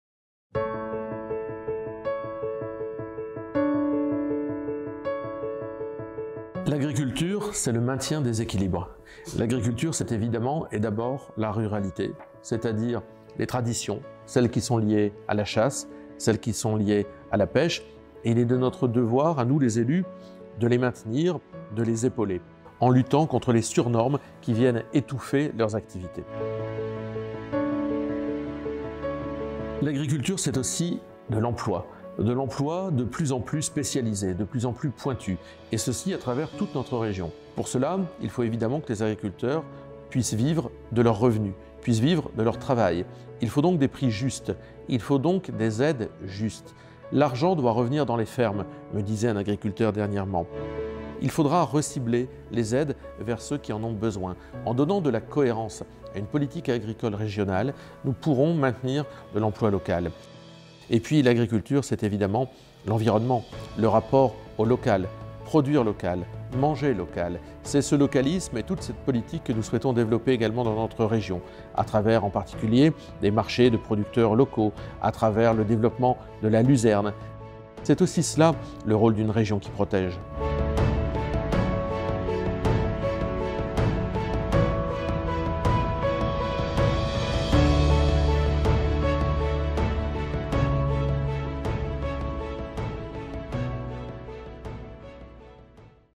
Défendre la ruralité dans les Hauts de France.
Cette semaine de mobilisation des Hauts-de-France a permis de mettre en valeur les formidables atouts de notre agriculture. A la tête de la région, nous défendrons le localisme, les traditions et le travail de nos paysans pour qu’ils aient enfin les revenus qu’ils méritent.